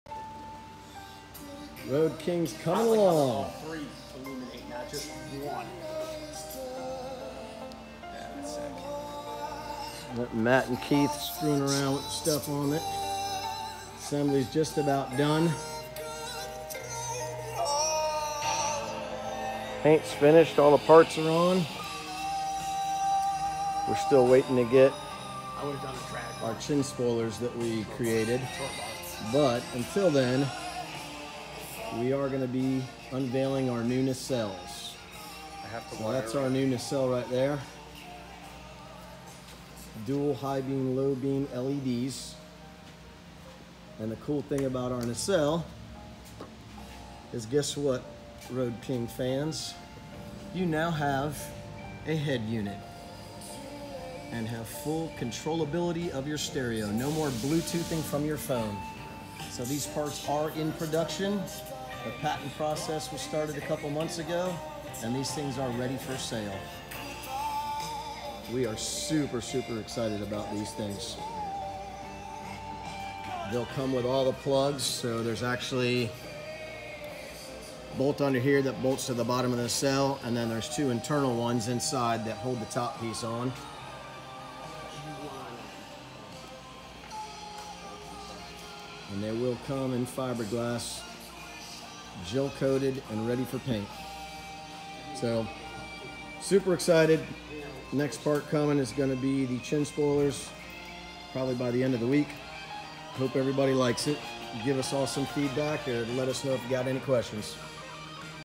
Check out our new Road King Nacelle
We've been working hard on developing this new nacelle. Now available for your Road King!
Custom paint by Eric Warren Artistry